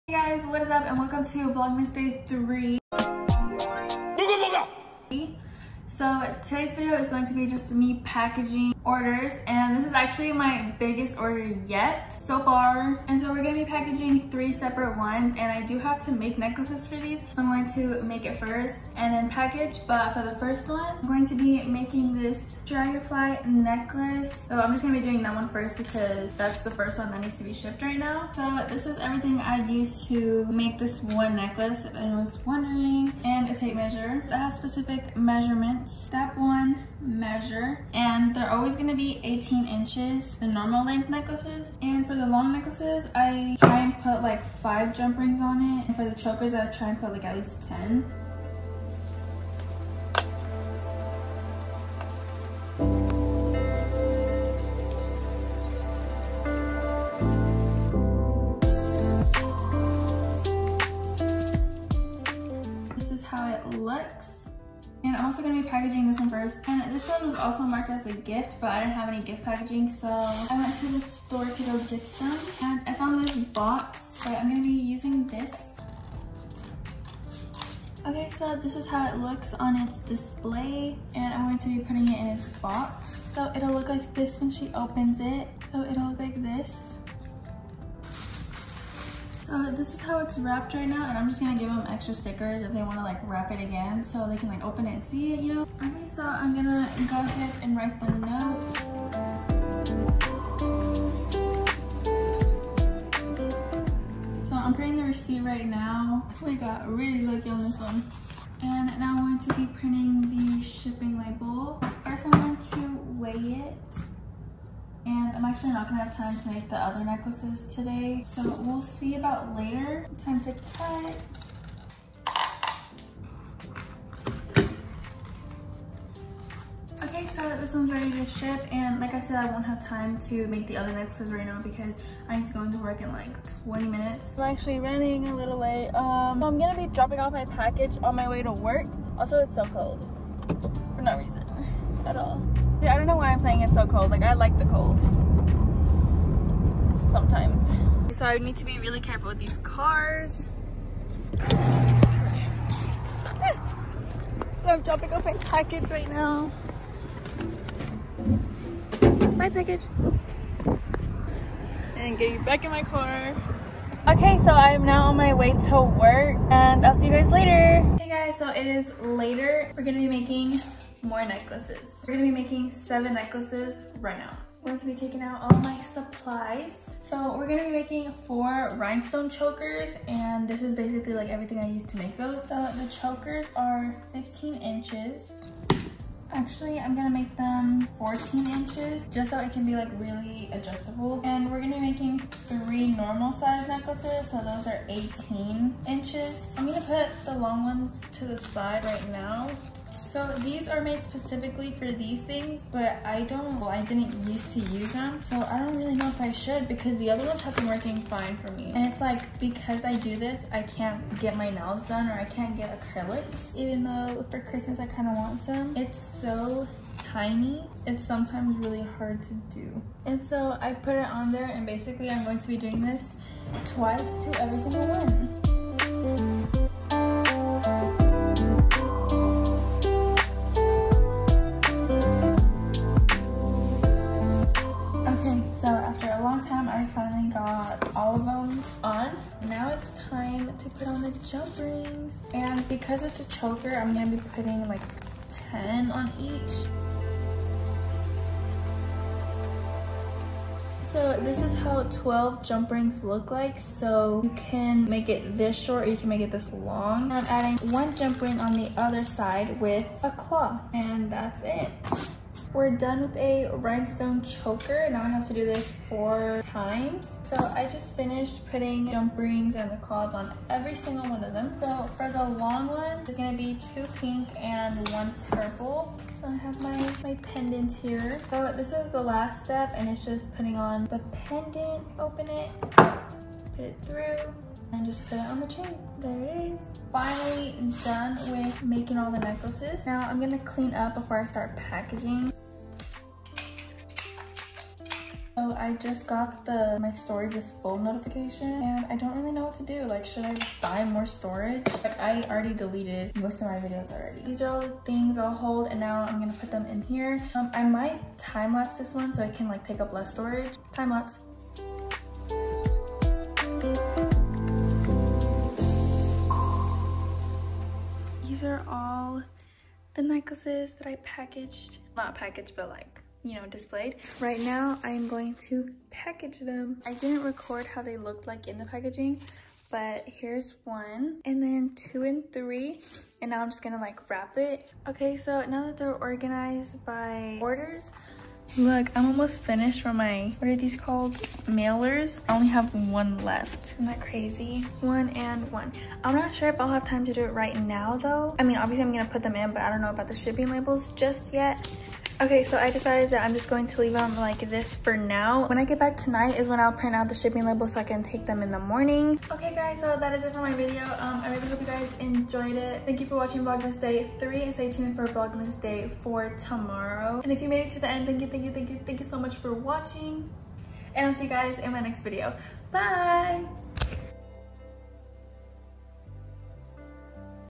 My biggest order yet!! Make & package necklaces with me | Vlogmas day 3
•socials

jewelry insta: brianasjewelry
personal insta: briana.coria

•last video

•music used

•sub count

2,836

video description:

in this video i’m packaging my etsy orders and i’m showing you guys how I make the necklaces I sell on etsy, and how I package my orders that are ready to ship! I havent had an order this big yet so it really meant a lot to me to make this video thank you for the support thank you so much 🤍🤍🤍